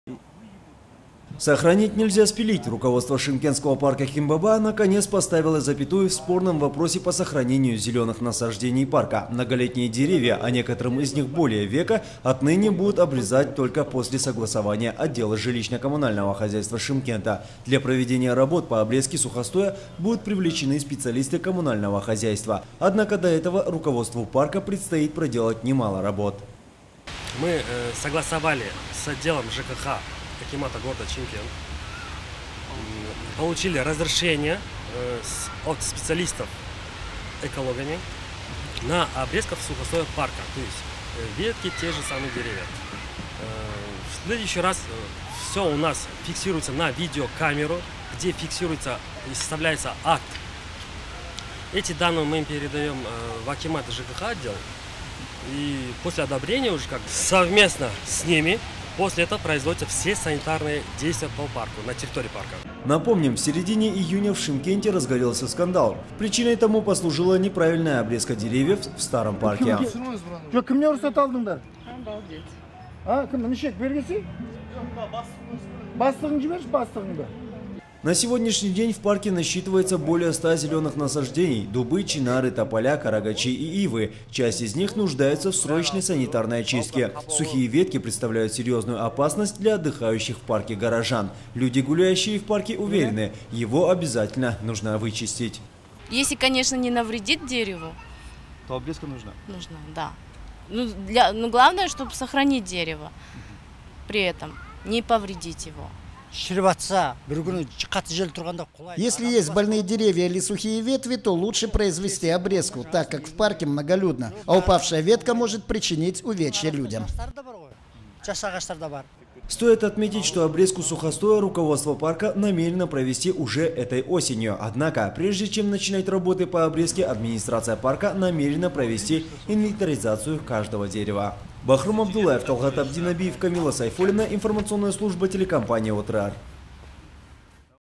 Деревья в парке "Кен-баба" будут обрезать только после согласования с экологами
Сохранить нельзя спилить. Руководство шымкентского парка Кен-баба, наконец, поставило запятую в спорном вопросе по сохранению зеленых насаждений парка. Многолетние деревья, а некоторым из них более века - отныне будут обрезать только после согласования с отделом ЖКХ Шымкента.